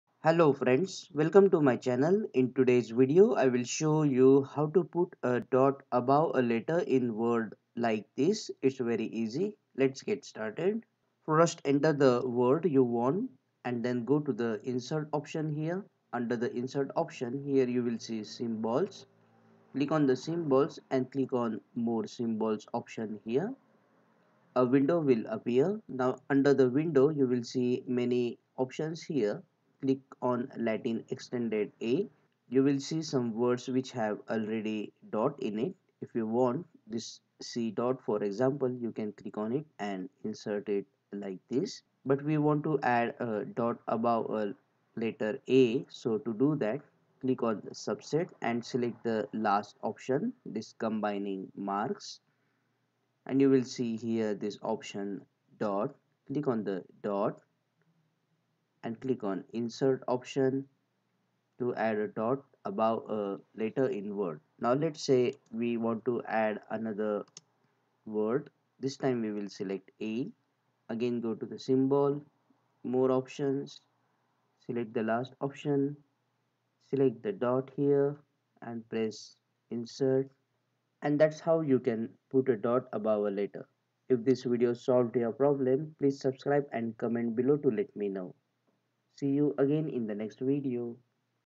How to Put a Dot Above A Letter in Word - [ Ȧ ȧ ] 😎
How to Put a Dot Above A Letter in Word is shown in this video. You can easily put a dot on top of a letter in word by using the insert symbol option and diacritical combination under subset